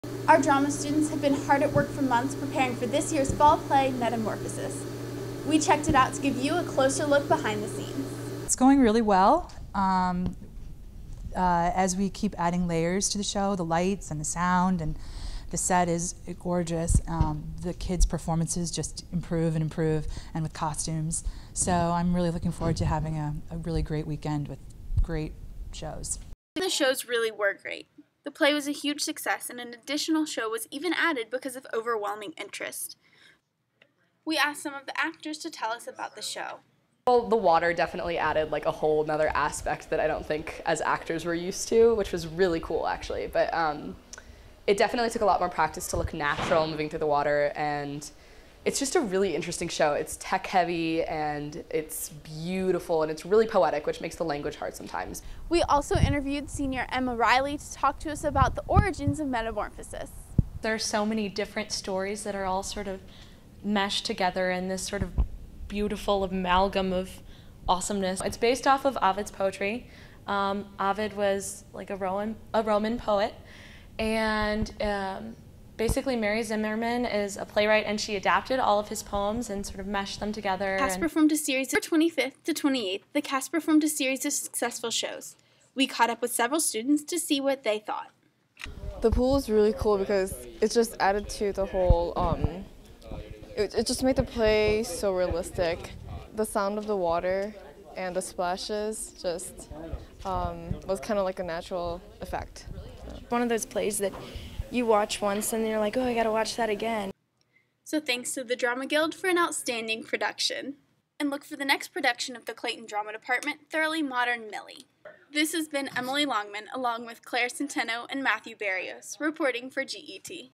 Clayton High School Fall Play: Metamorphosis
A video recap of Clayton's production of Mary Zimmerman's "Metamorphosis," created for an episode of GET in the fall of 2012.